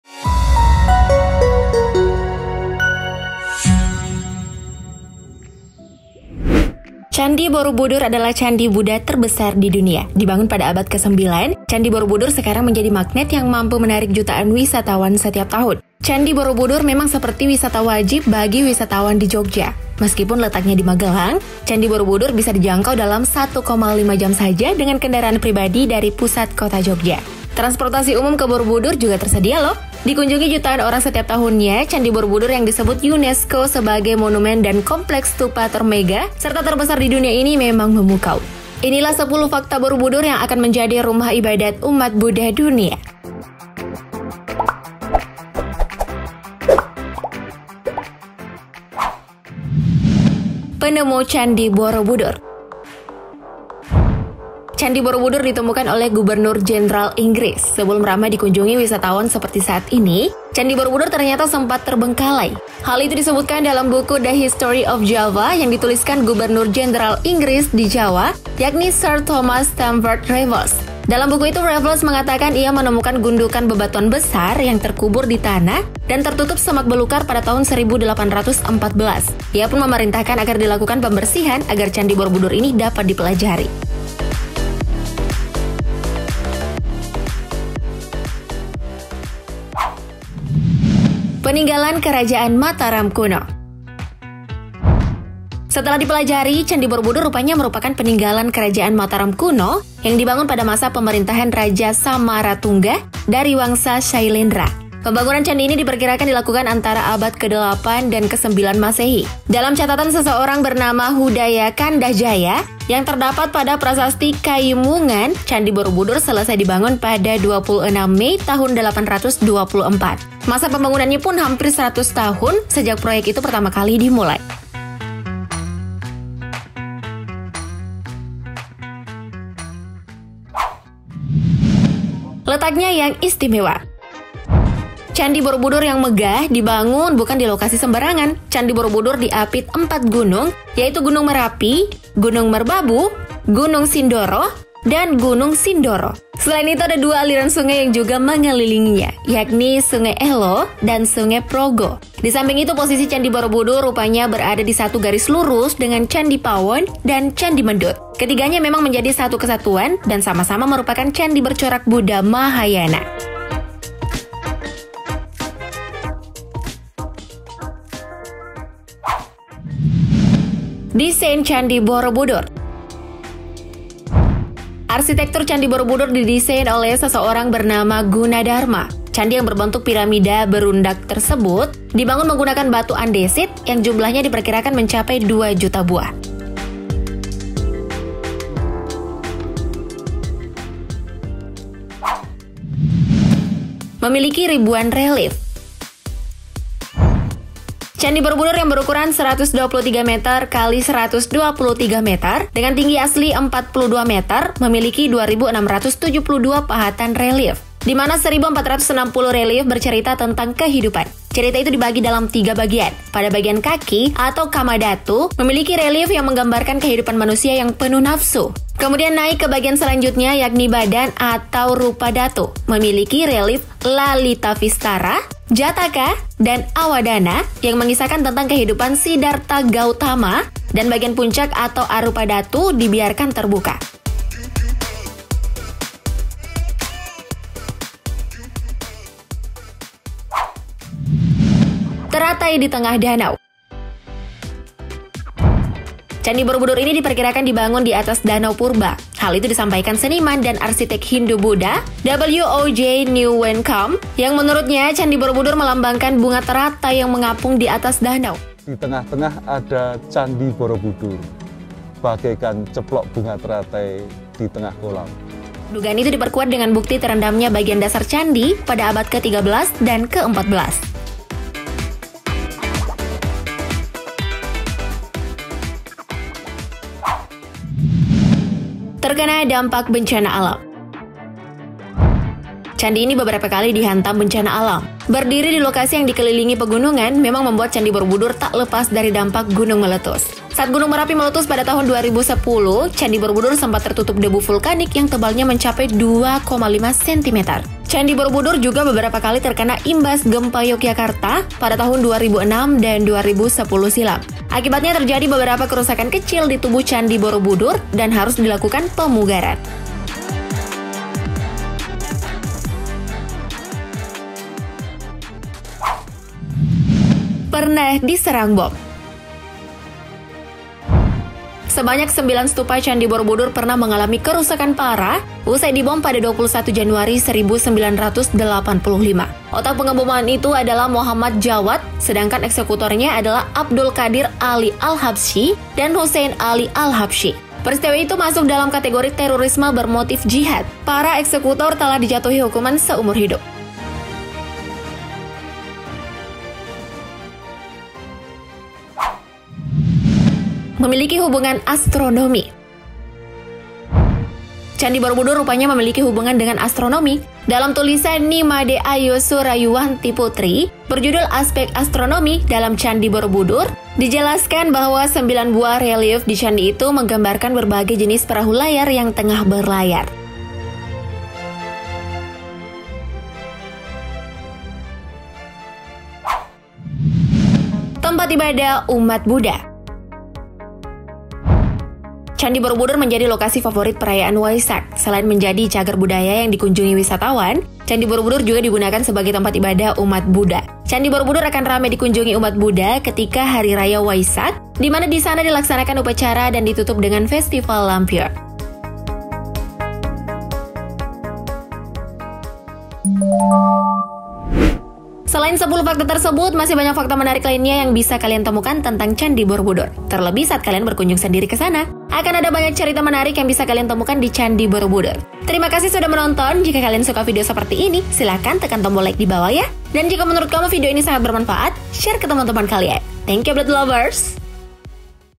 Diapit Empat Gunung Merapi, Ternyata Candi Borobudur Dibangun Diatas Danau
Candi Borobudur adalah candi Buddha terbesar di dunia. Dibangun pada abad ke-9, Candi Borobudur sekarang menjadi magnet yang mampu menarik jutaan wisatawan setiap tahun. Candi Borobudur memang seperti wisata wajib bagi wisatawan di Jogja. Meskipun letaknya di Magelang, Candi Borobudur bisa dijangkau dalam 1,5 jam saja dengan kendaraan pribadi dari pusat Kota Jogja. Transportasi Umum ke Borobudur juga tersedia loh!

Dikunjungi jutaan orang setiap tahunnya, Candi Borobudur yang disebut UNESCO sebagai monumen dan kompleks stupa termegah serta terbesar di dunia ini memang memukau. Inilah 10 Fakta Borobudur yang Akan menJadi Rumah Ibadat Umat Buddha Dunia.